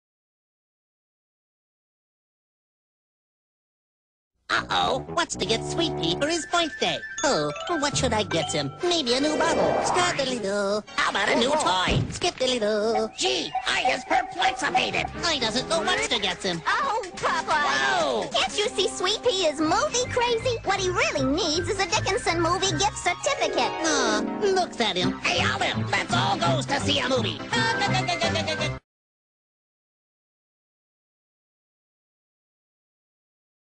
Sonny Melendrez as voice of Popeye for Dickinson Movie Theaters
Sonny Melendrez as Popeye. Produced by Bill Reed.